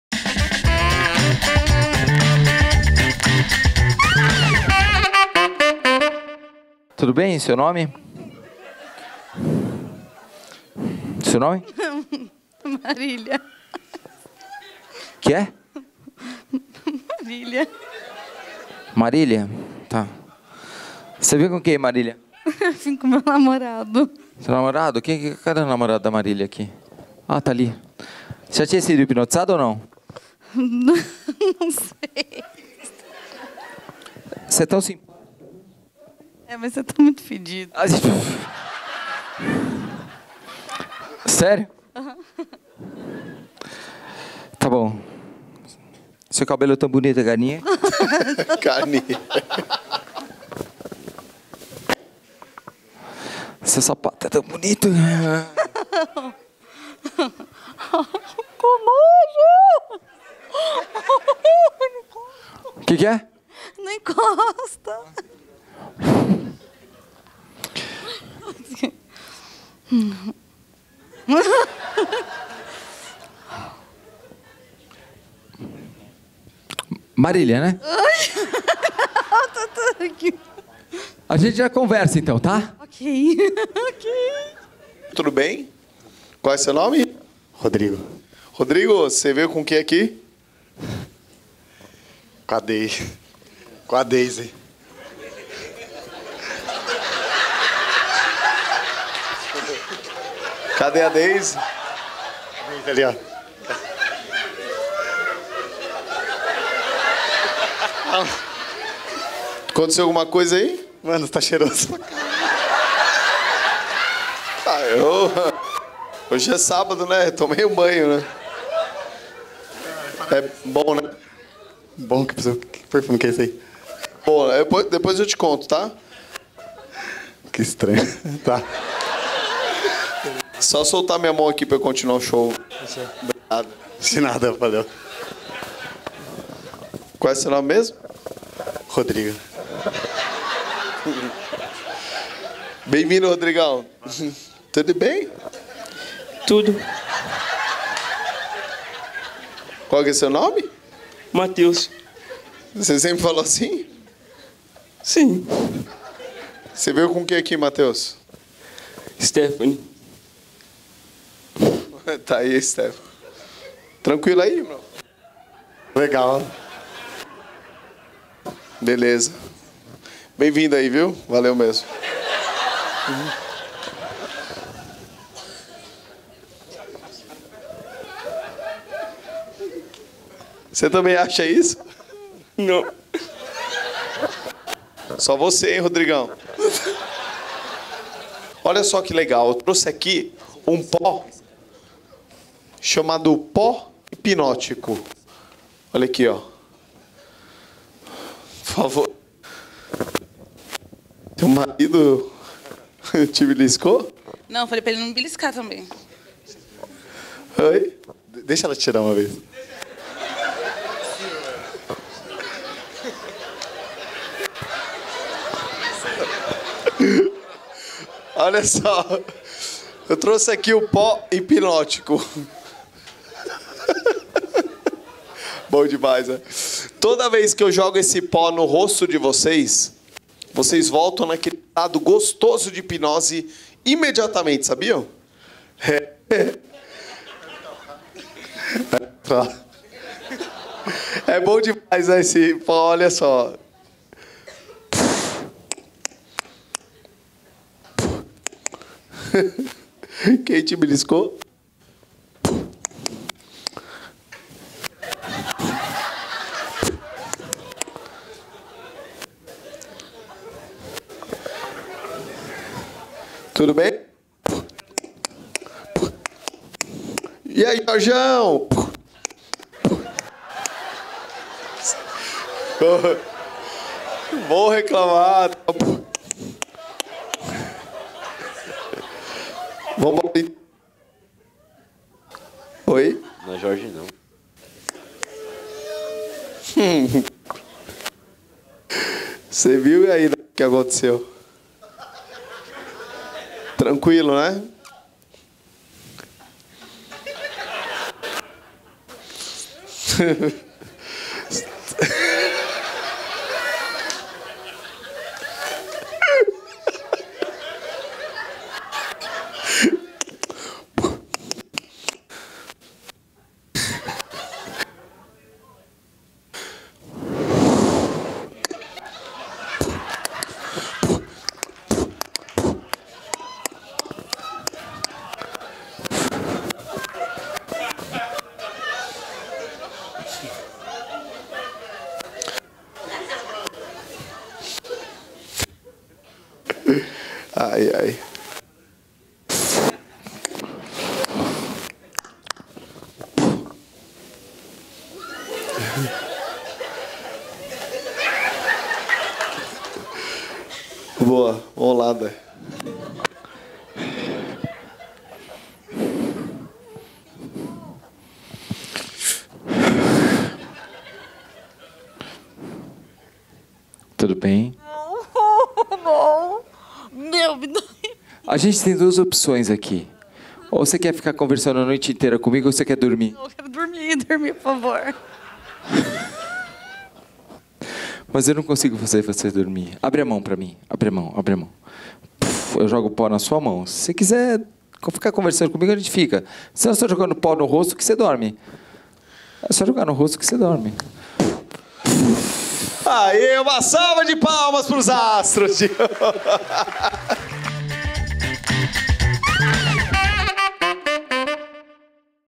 HIPNOSE - VEJA PORQUE ELA "DORMIU" FELIZ (27/10/18)
Em nossa apresentação do dia 27/10, uma de nossas participantes "dormiu" ao final. E ela "dormiu" feliz :D

2018 - Janeiro à Abril - Todos Sábados às 20h